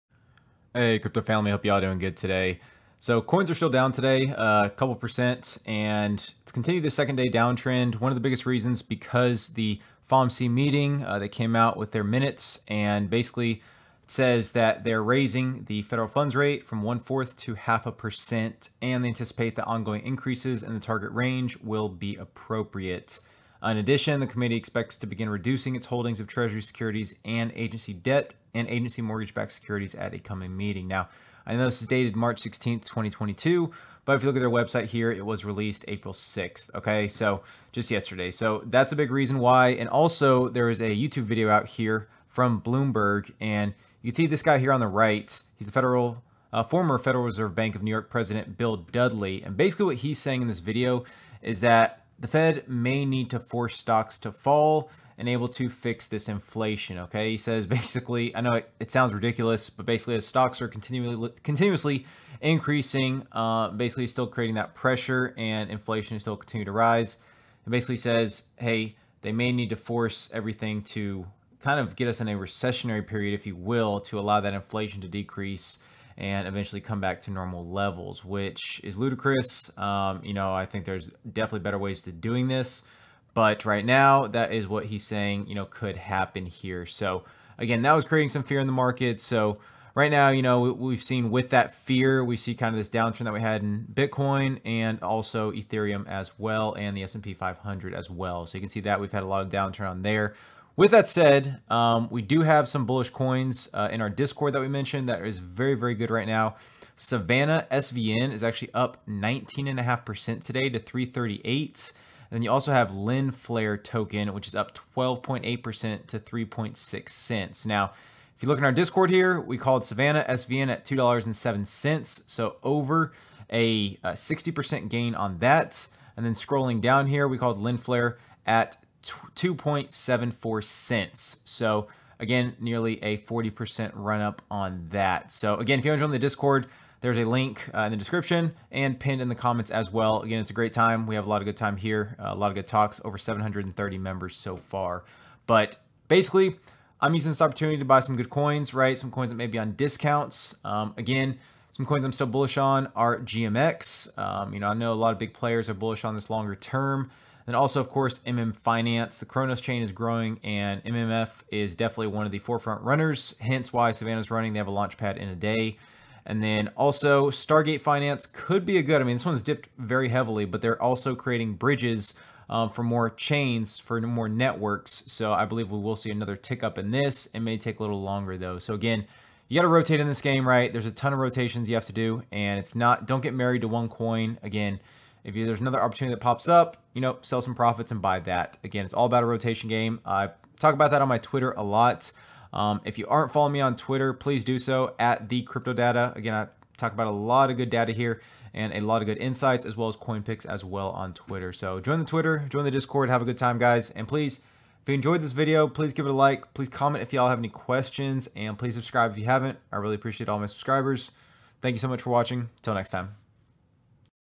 Why is The Market Declining? SVN and LFT Outperforming!
In this video I cover the reason for the recent market dip and what coins are outperforming! 🙏